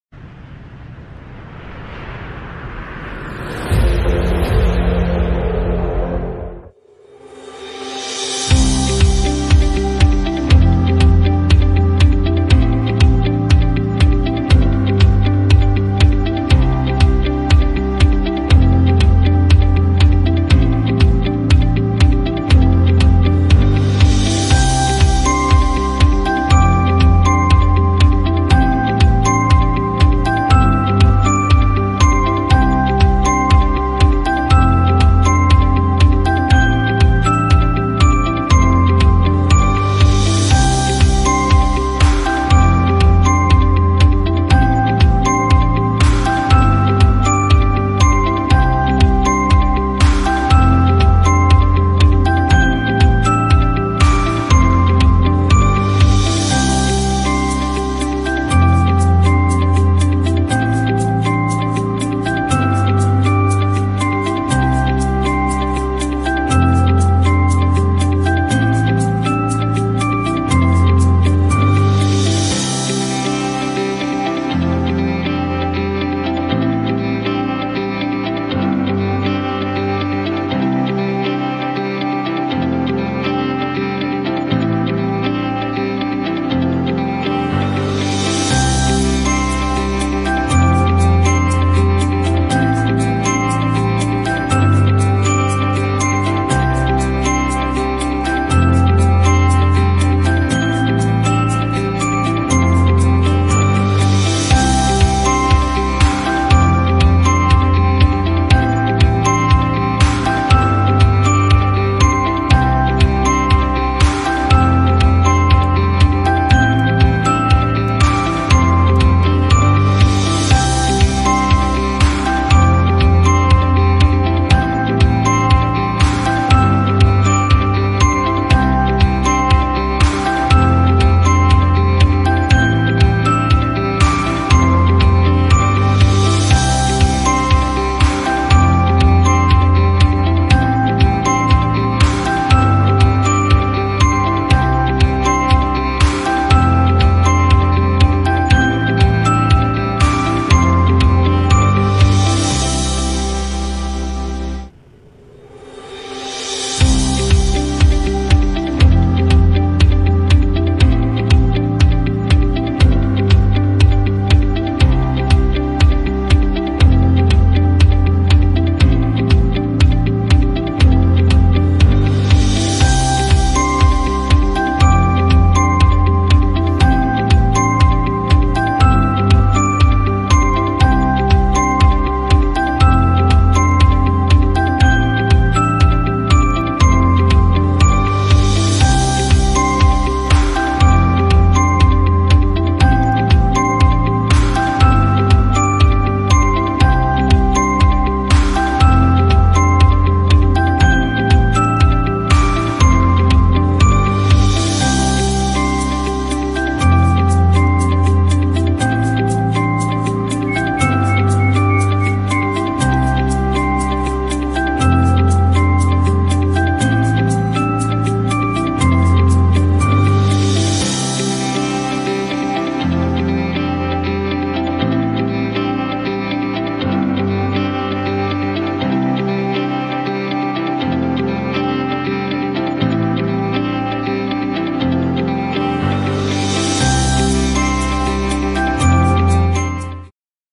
CNFSimulator.FTD.D40/D42, Diamond DA40/DA42 switchable level 5 FTD
The DA40/DA42 FTD is with highly convenient. The entire aircraft adopts a modular design, which is convenient for quick maintenance and software upgrades. It can also quickly switch between DA40 and DA42 models, saving you time and economic costs. Moreover, this set of quick switch solution can fully meet the certification requirements of the Civil Aviation Administration for Level 5 FTD.